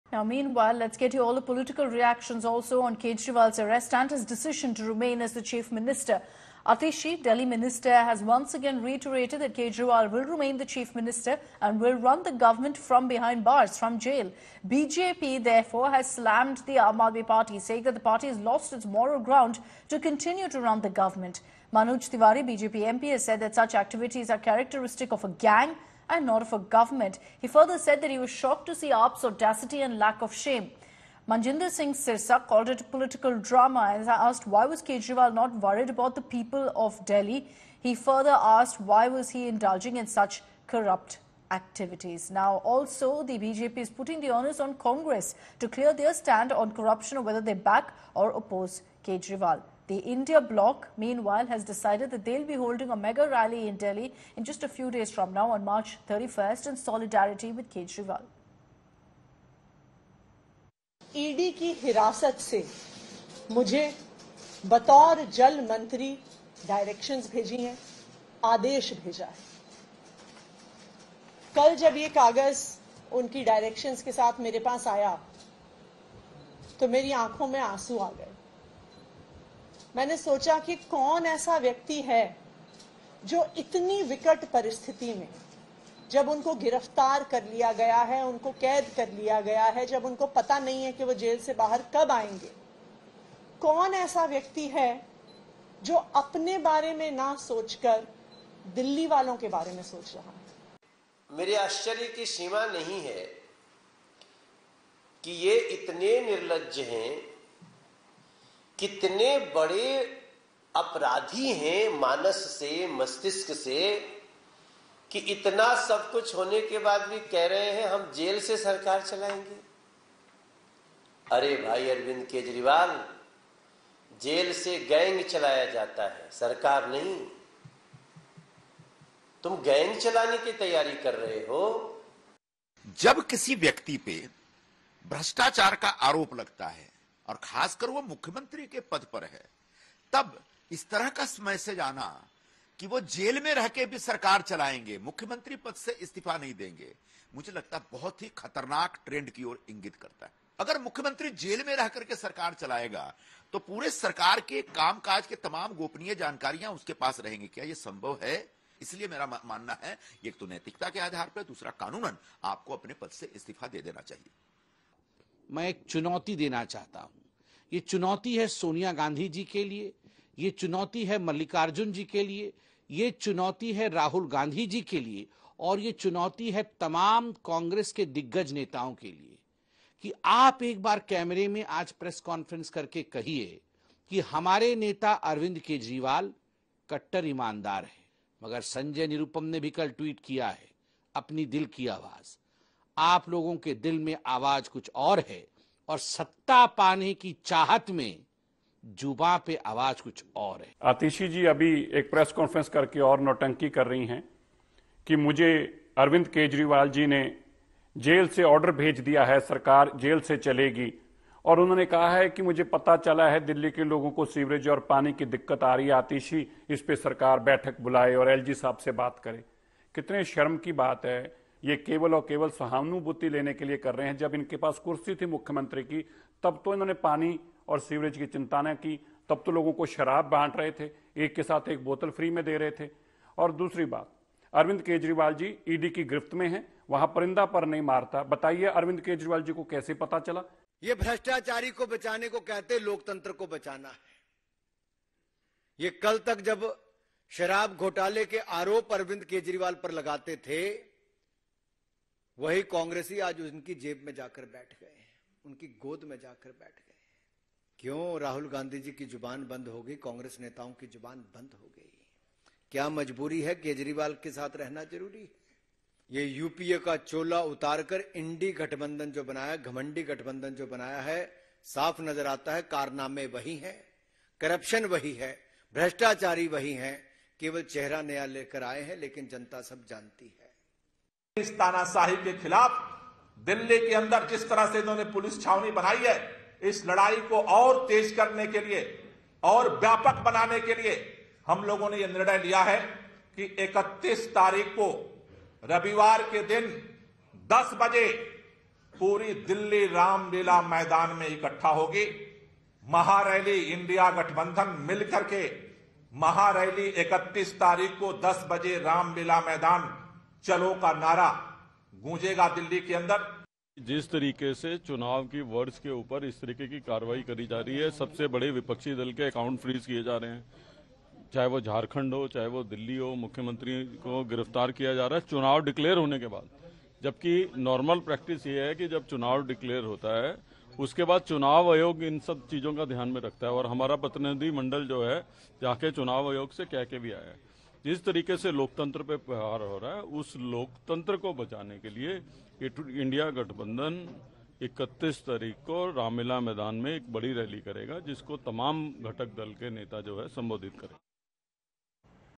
Kejriwal's Arrest Sparks Political Uproar: Will Continue as Chief Minister from Jail
In a major political development, Delhi Chief Minister Kejriwal's arrest has stirred a storm in the political arena. Despite his arrest, he has decided to continue his duties as the Chief Minister from jail, a move that has drawn criticism from the BJP.